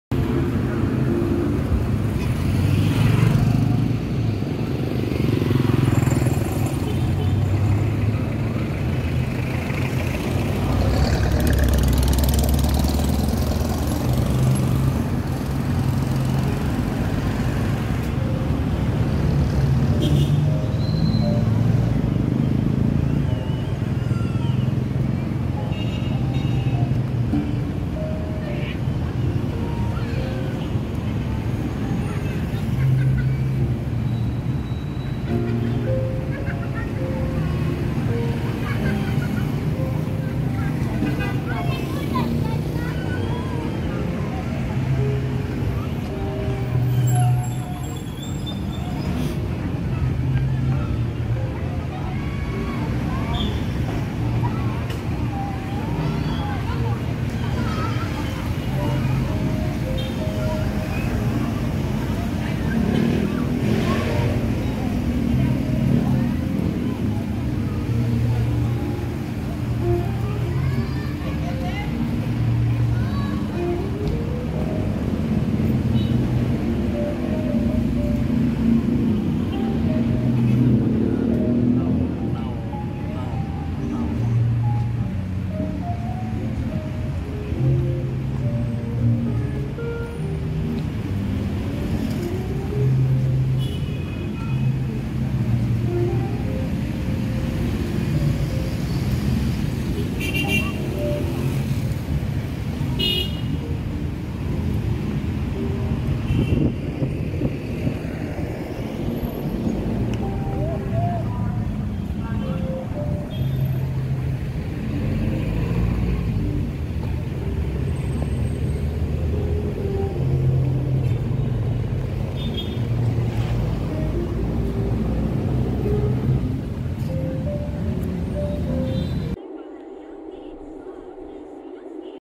Jaro Plaza I Iloilo City I Philippines I August 8 2023
just walk in jaro plaza area